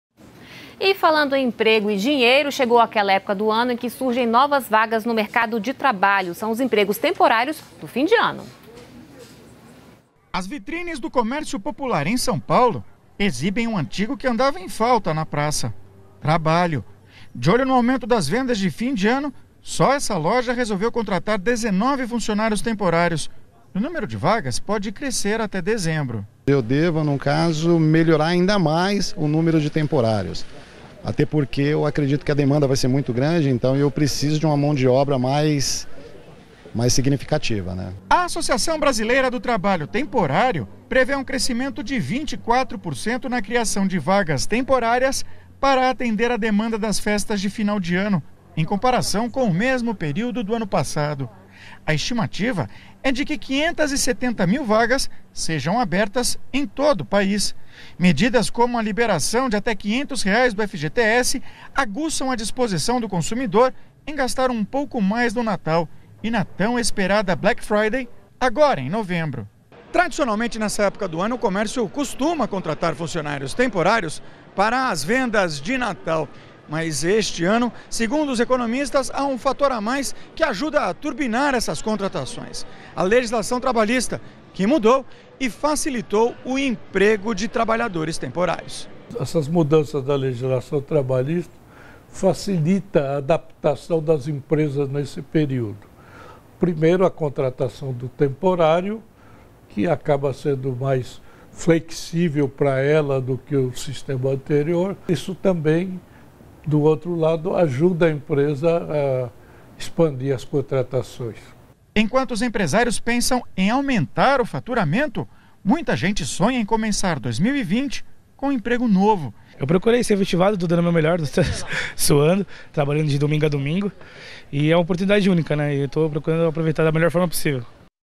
Empregos temporários devem aumentar este ano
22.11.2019 - Legislação trabalhista mais flexível e liberação do FGTS devem aquecer mercado e aumentar demanda de consumo. A previsão é de crescimento na geração de empregos temporários.